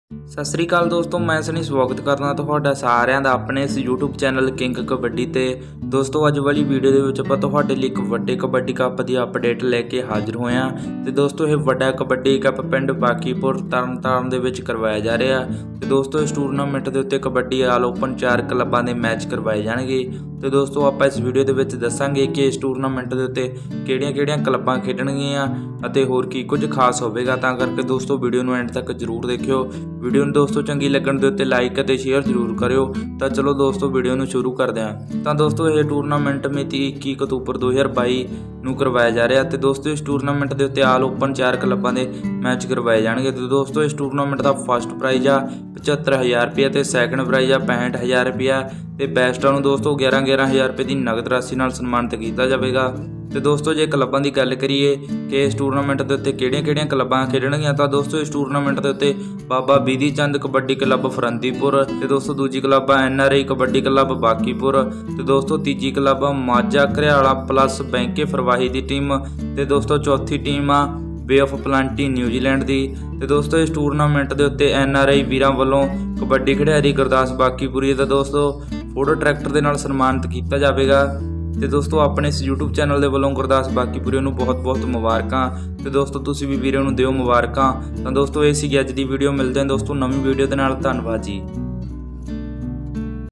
ਬਾਕੀਪੁਰ ਹੋਵੇਗਾ ਵੱਡਾ ਕਬੱਡੀ ਕੱਪ ਚਾਰ ਆਲ ਓਪਨ ਕਲੱਬਾਂ ਦੇ ਮੈਚ ਹੋਣਗੇ/Live Kabaddi match/Usa Kabaddi cup fanl
Hi
       I am sunny khurana Welcome to our You Tube Channel King Kabaddi

     About this video :-ਇਸ ਵੀਡੀਓ ਦੇ ਵਿਚ ਬਾਕੀਪੁਰ ਤਰਨ ਤਾਰਨ ਦੇ ਕਬੱਡੀ ਕੱਪ ਦੇ ਬਾਰੇ ਜਾਣਕਾਰੀ ਦਿੱਤੀ ਗਈ ਹੈ